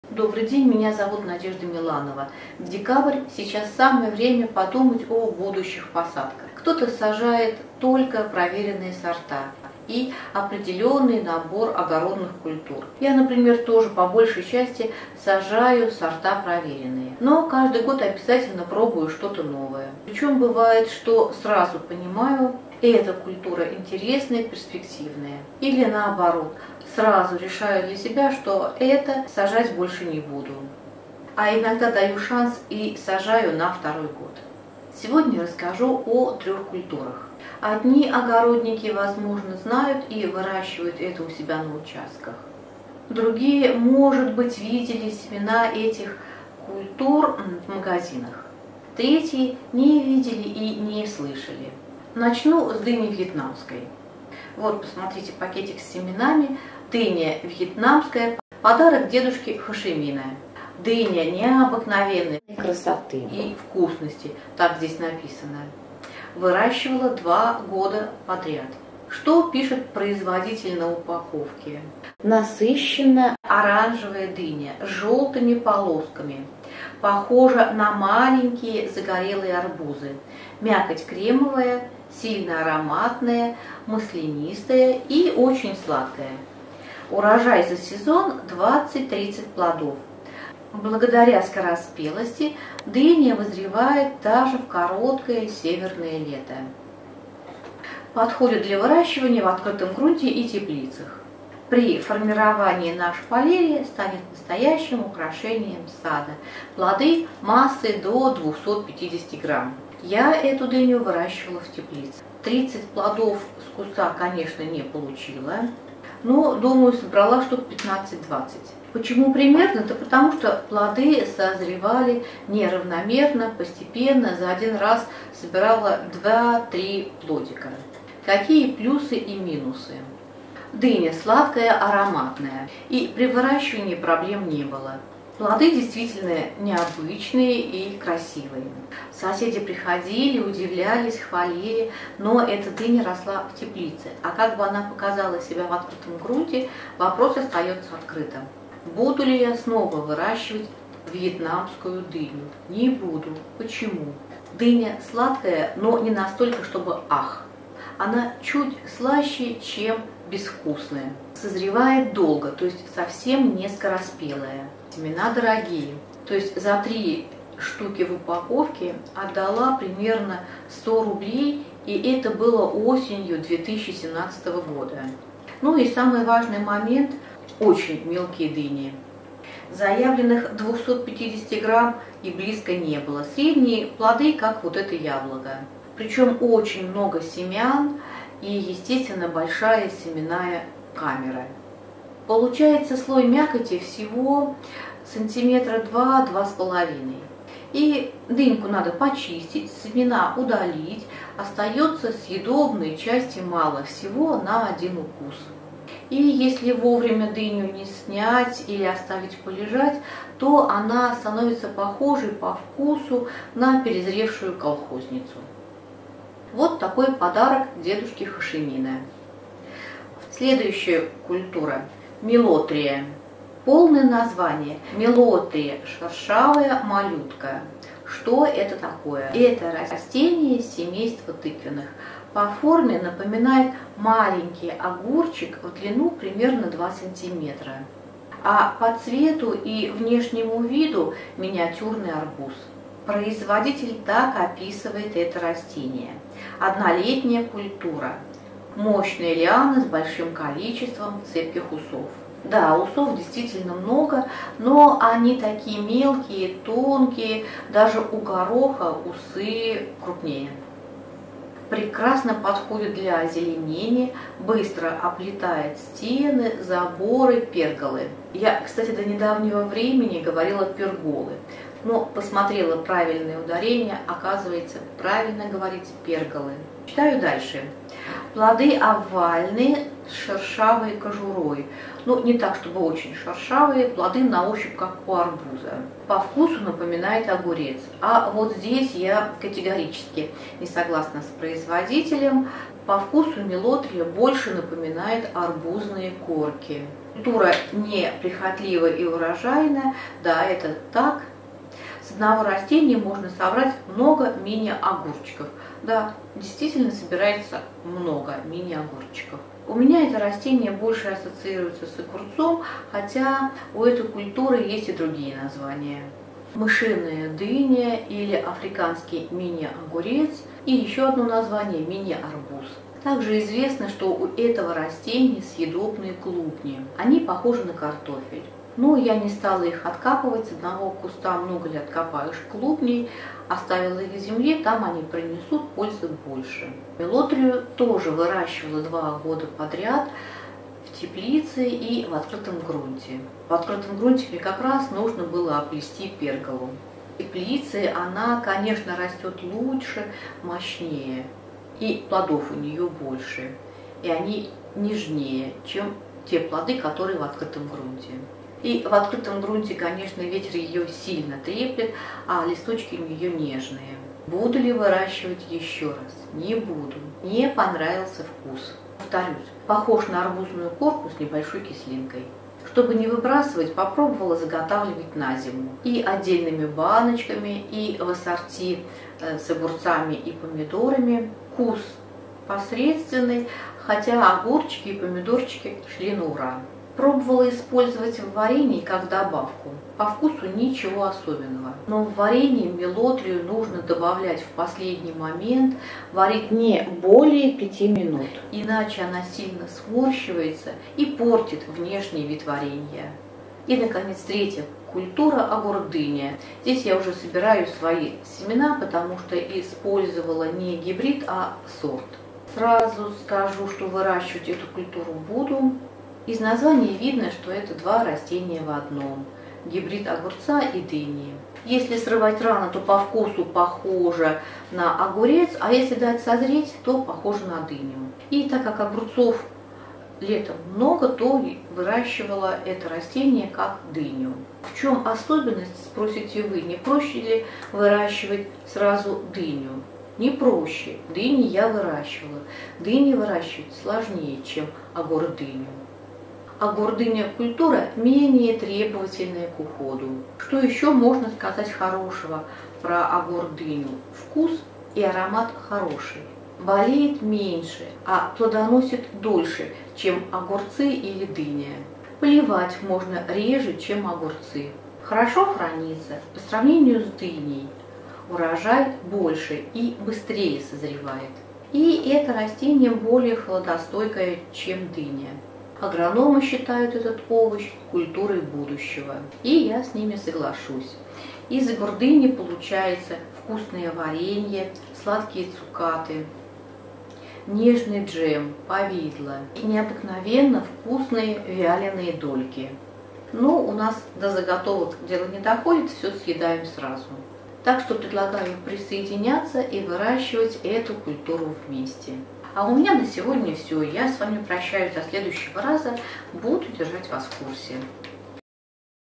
175_Давайте знакомится... Дыня вьетнамская. Дыня мышиная. Огурдыня.
Сейчас, в декабре, самое время подумать о будущих посадках.
Сегодня расскажу о трех, не очень распространенных культурах.  Эти культуры выращивала. Поделюсь своими впечатлениями.
Дыня вьетнамская. Подарок дедушки Хо Ши Мина.
Милотрия (дыня мышиная). Мелотрия шершавая Малютка.
Огурдыня.